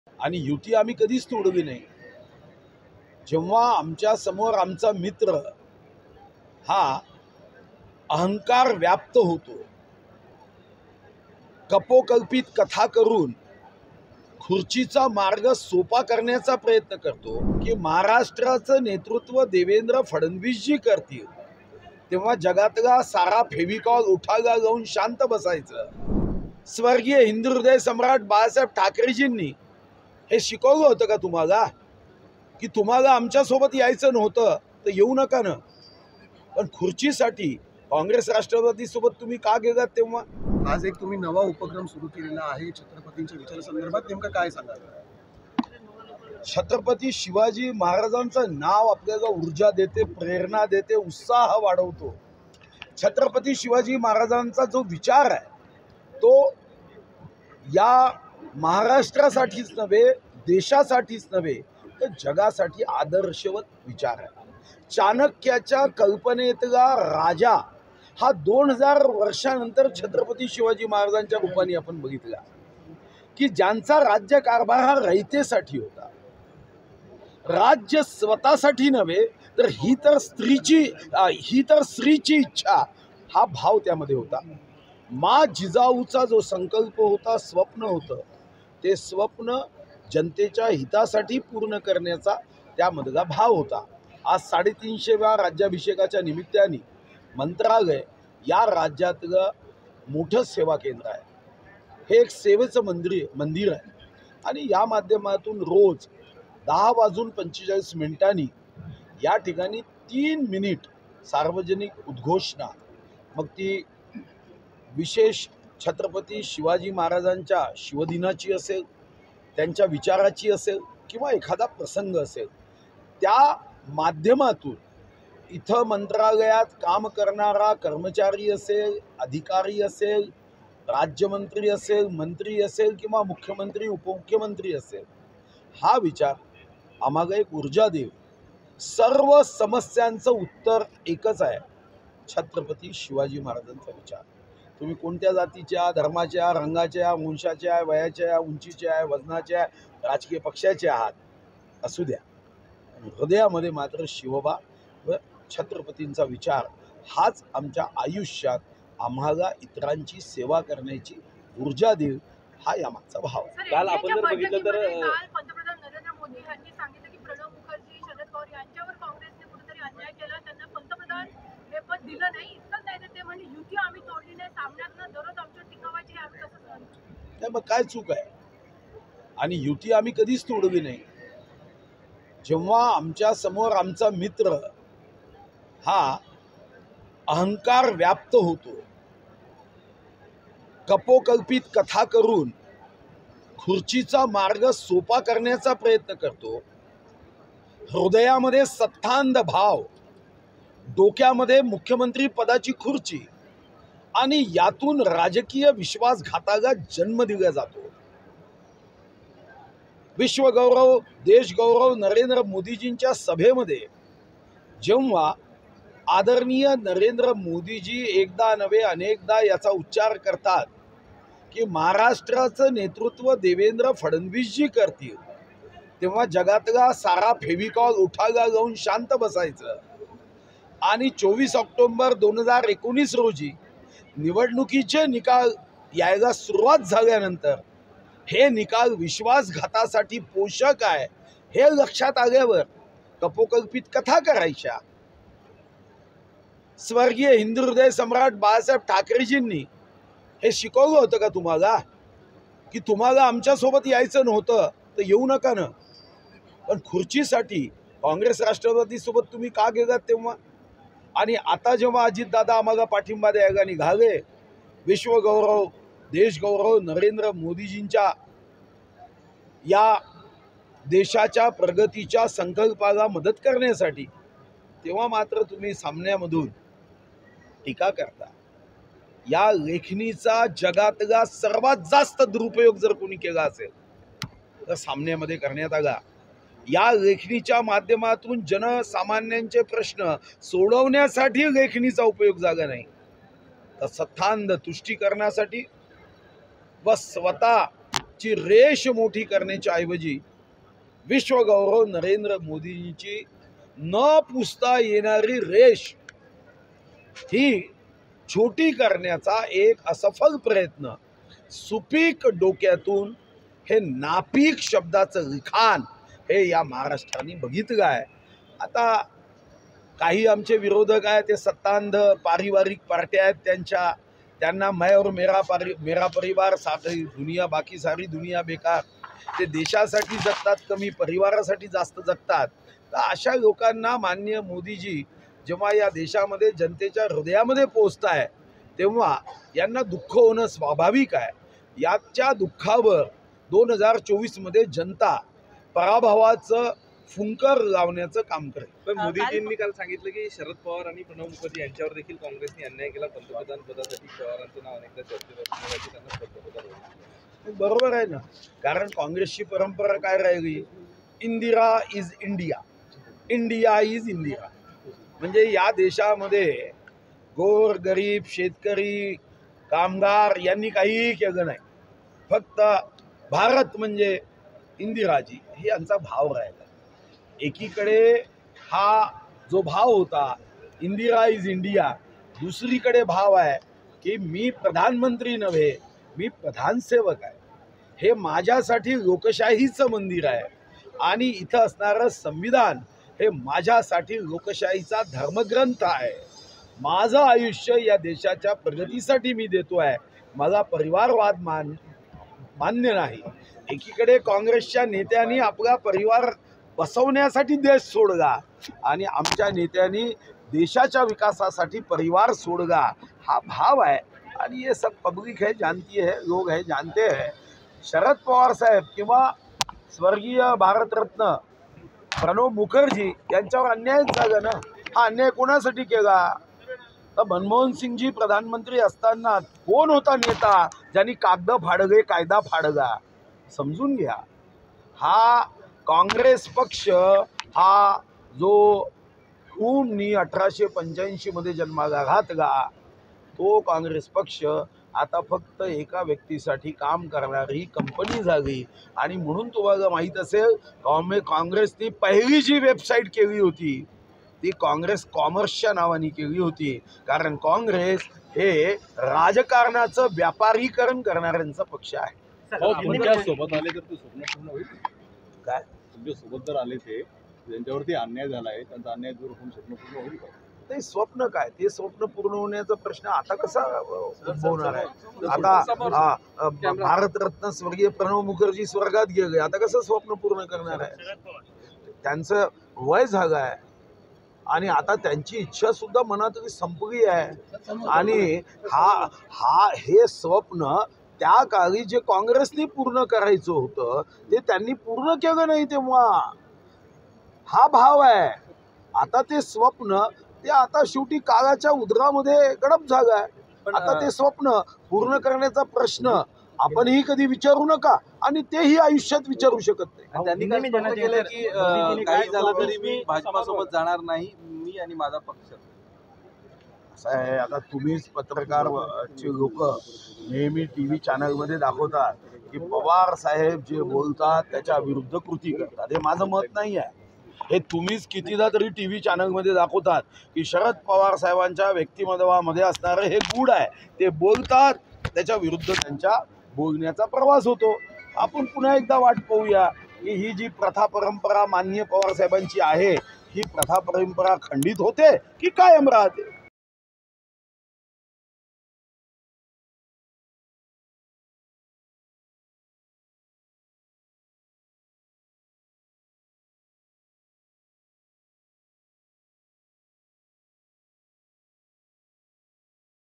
Sudhir Mungantiwar : सुधीर मुनगंटीवार यांचा उद्धव ठाकरेंवर घणाघात | LetsUpp Marathi
राज्याचे वनमंत्री सुधीर मुनगंटीवार यांनी उद्धव ठाकरे यांच्यावर घणाघाती टीका केली. उद्धव ठाकरेंनी सत्तेसाठी मुख्यमंत्रिपदाच्या खुर्चीसाठी भाजपशी दगाबाजी केल्याचा आरोप केला.